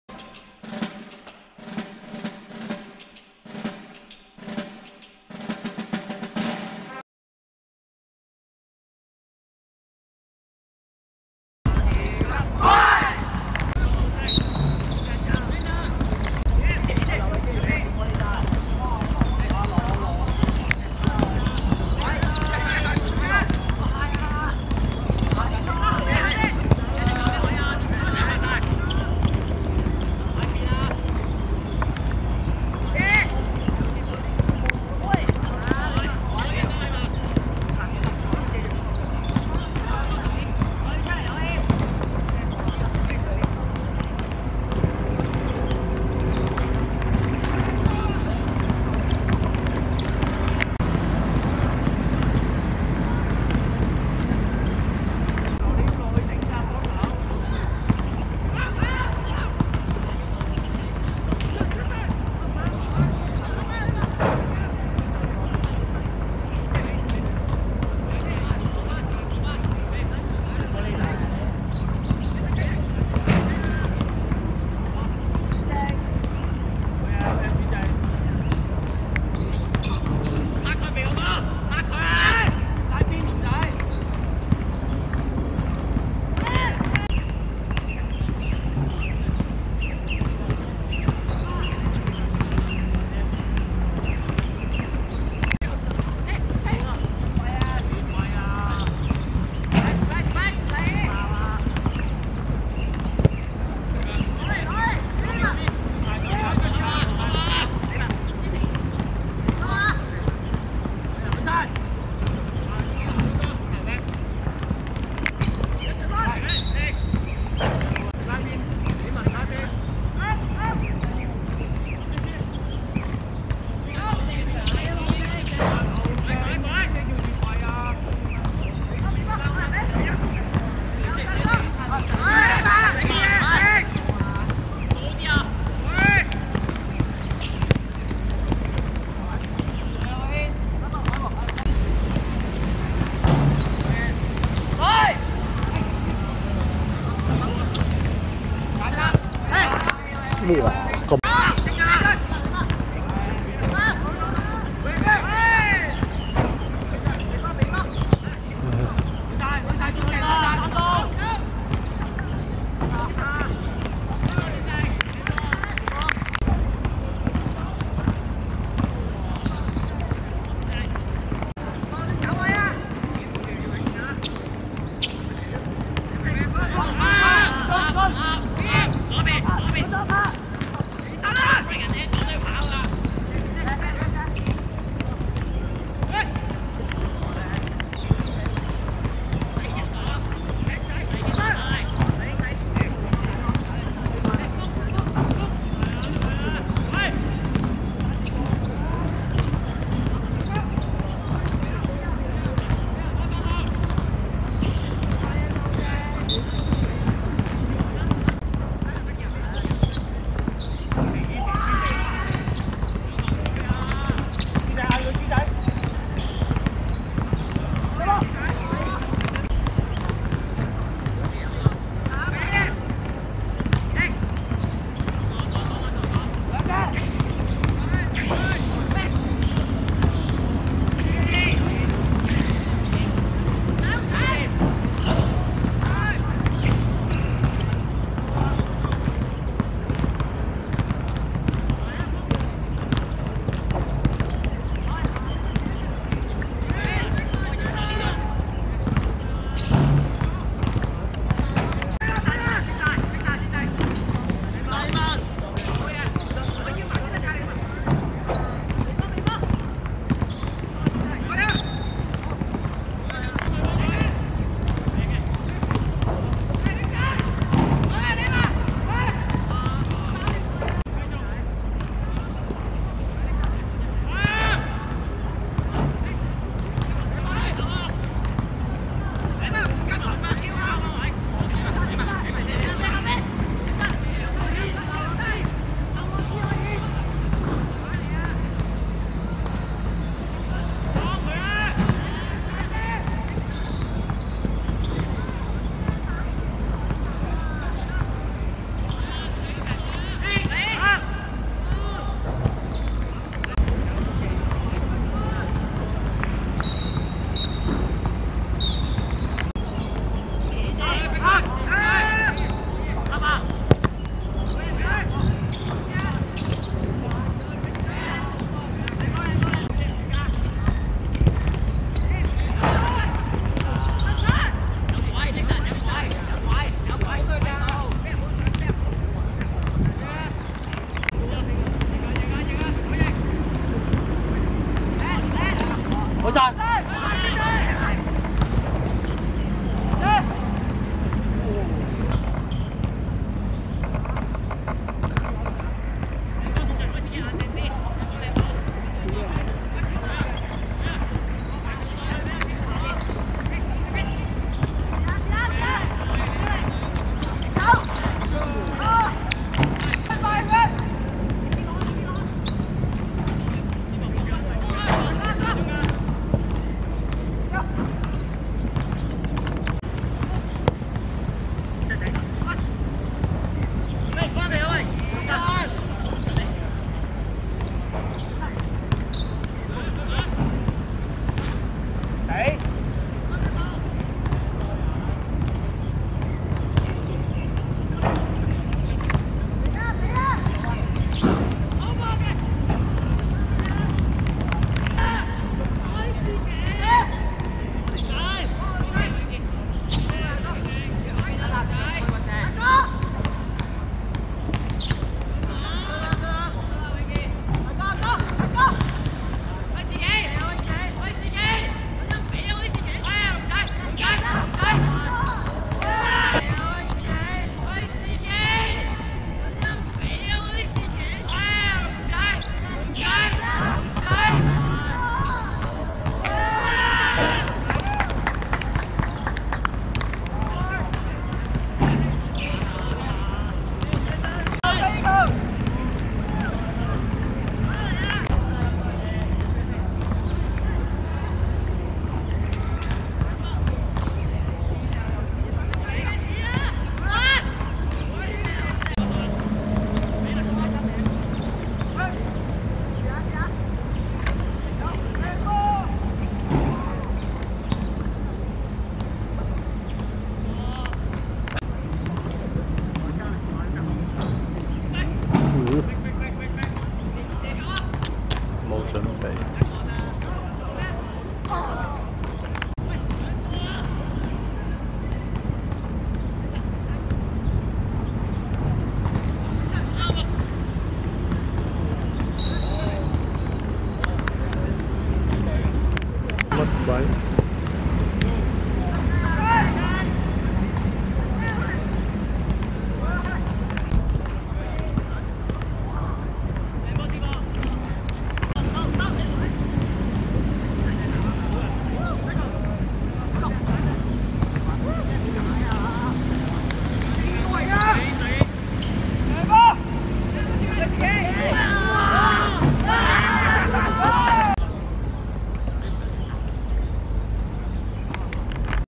可藝vs伊中舊生會(2016.8.13.新界西學界足球熱身賽分組賽)精華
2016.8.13.屯門湖山仿真草塲 09:30 可藝(綠) 1 : 0 伊中舊生會(藍)